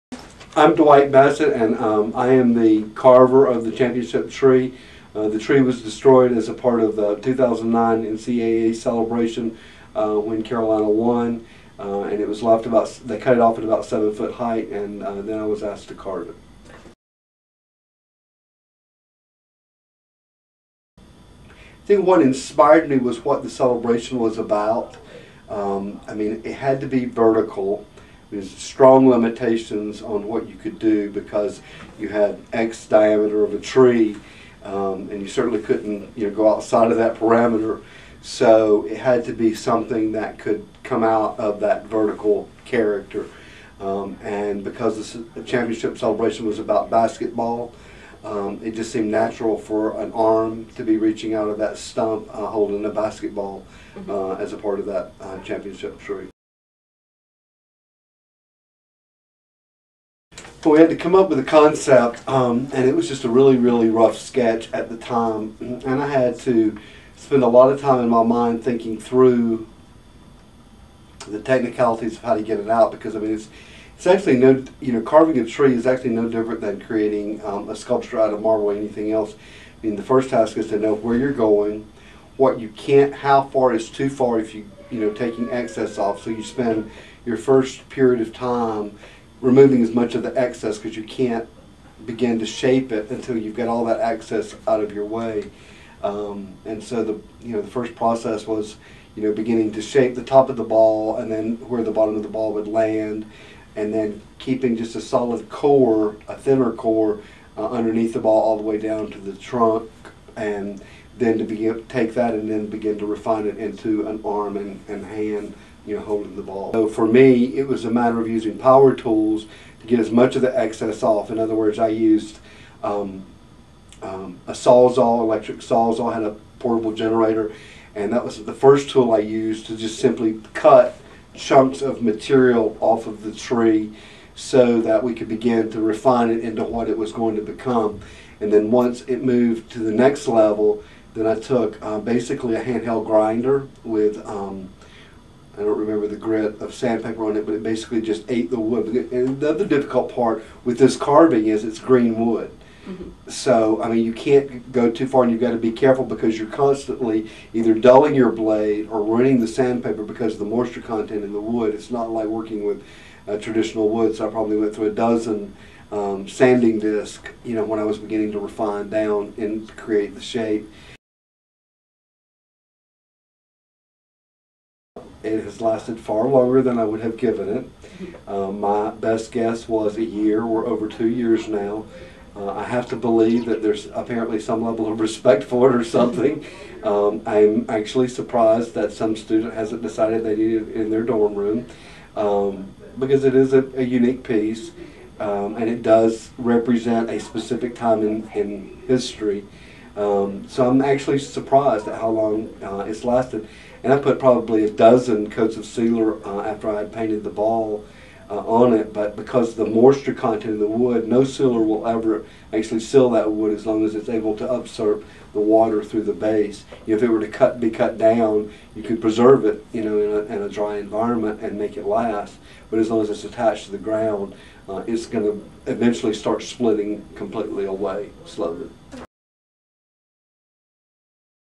ChampionshipTreeCompleted.mov
Title: Championship Tree; Artist: Dwight Bassett; Year Completed: 2009; Materials: Carved Live Ash

Championship Tree is a tribute to the UNC-Chapel Hill Tar Heels Men's basketball Team and its 2009 NCAA championship.

The tree outside Spanky's restaurant, at the intersection of Franklin and Columbia streets was damaged during the street celebration that ensued after the UNC-Chapel Hill Tar Heels Men's Basketball Team won the NCAA Championship.  Mayor Kevin Foy had the idea to make art out of the remaining stump.